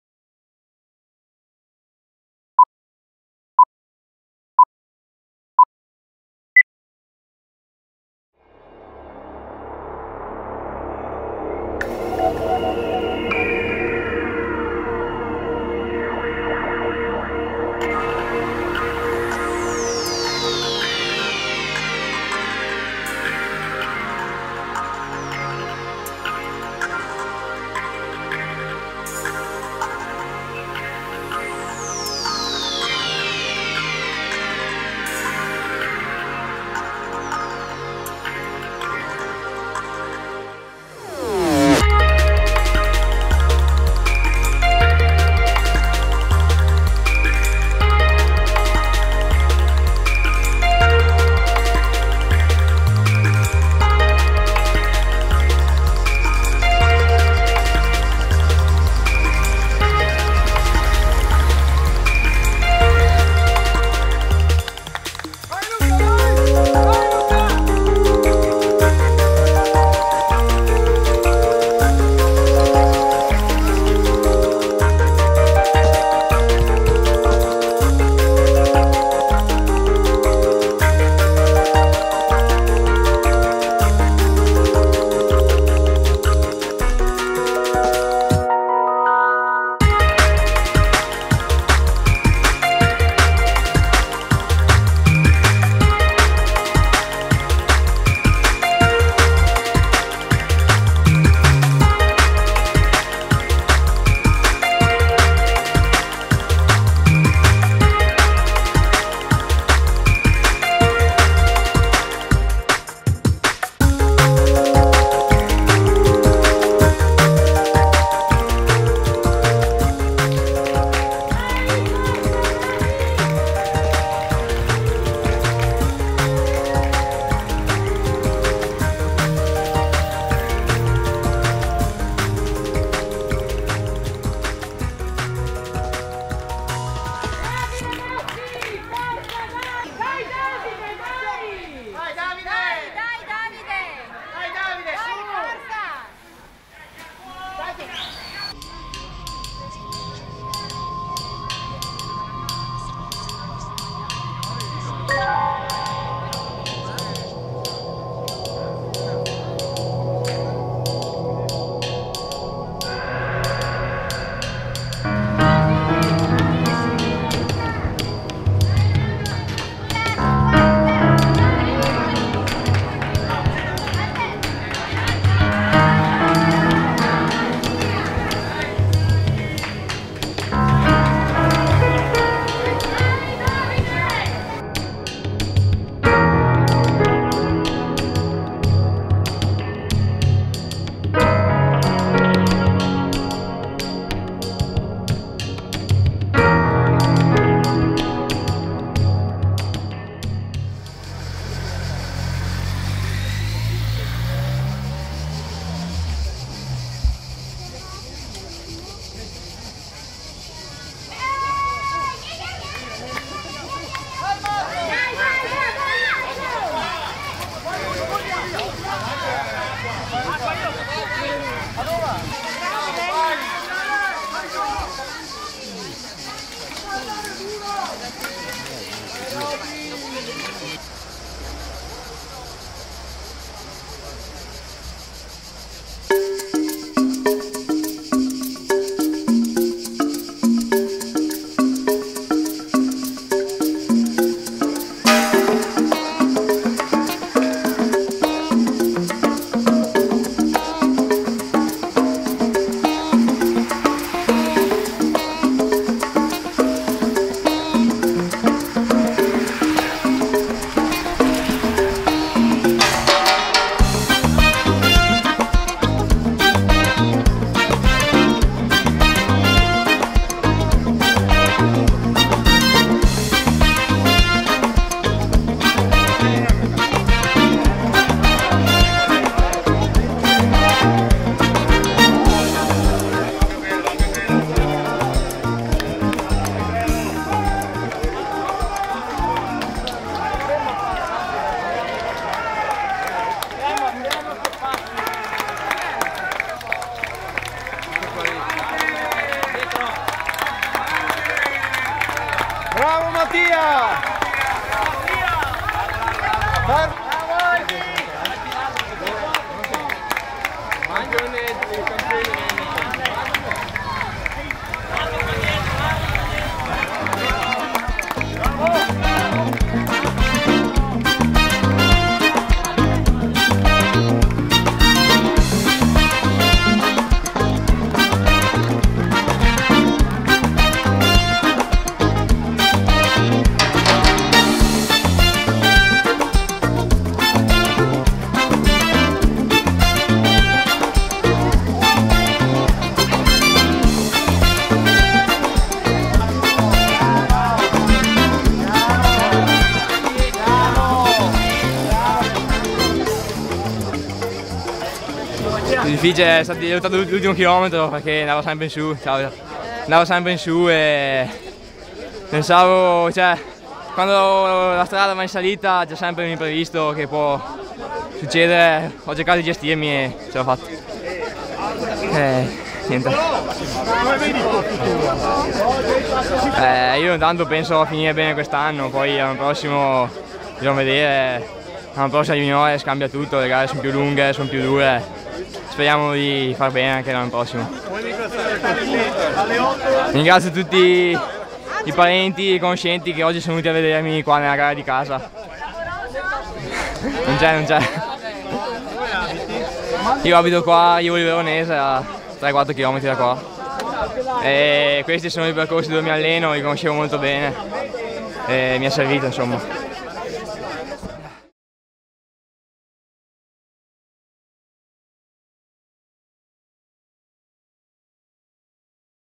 Coppa Città di Cavaion - Gara Ciclistica per Allievi
52° Coppa Città di Cavaion
Gara Ciclistica categoria Allievi F.I.C. - VIII Trofeo EFFEGI.
Video della gara ciclistica corsa sui dolci declivi del territorio del Comune di Cavaion, in Provincia di Verona, domenica 21 luglio 2013 in occasione della 283° Festa della Madonna del Carmine.
Le riprese sono state effettuate dalla casa di produzione Belle Epoque Film, specializzata anche in video di ciclismo.